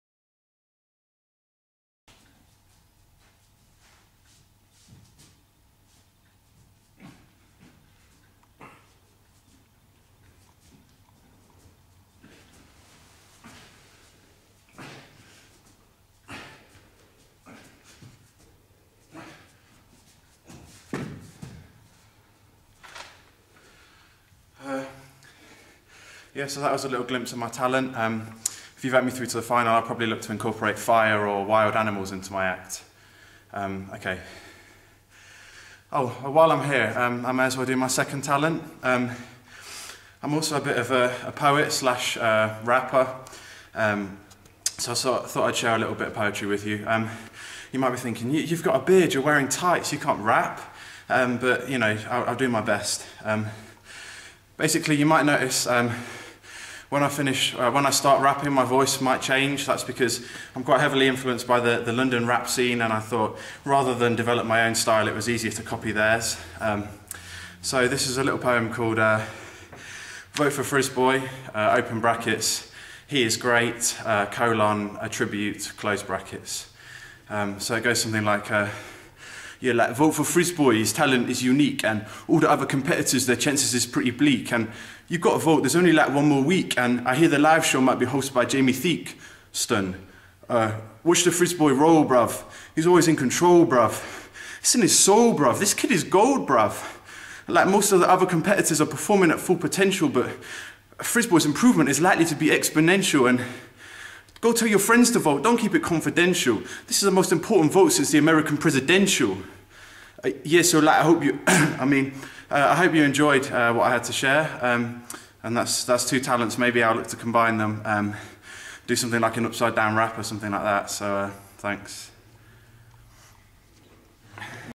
UCLan's Got Talent: 'The Upside Down Rapper' (Vote for Frisboy)
what a talented fellow